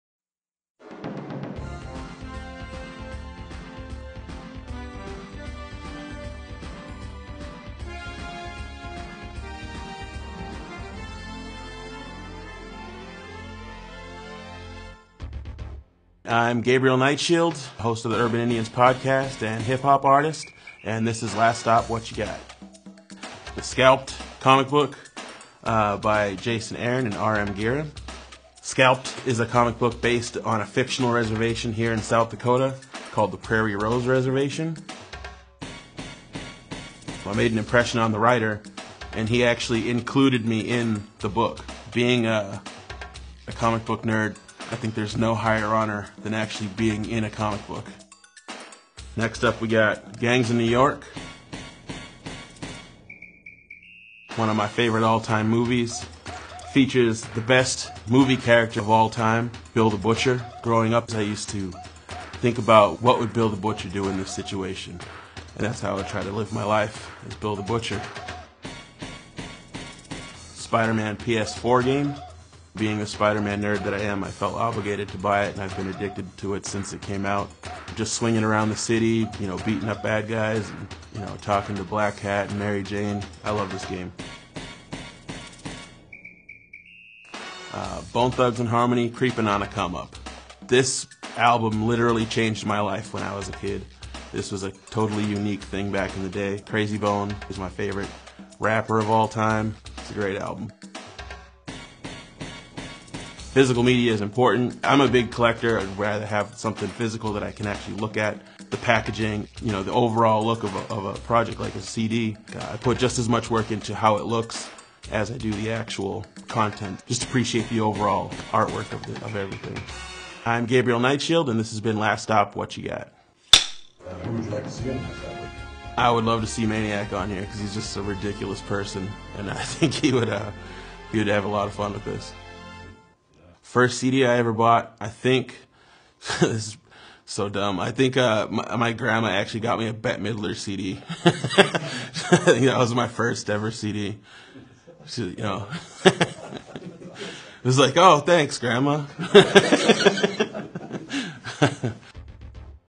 Last Stop Whatchya Got-Episode-21 featuring Gabriel Night Shield
Happy Friday folks!  We have another episode of LSWG for you.  Local hip hop genius Gabriel Night Shield shows us what he found at Last Stop!  We had a blast hanging with Night Shield and making this video.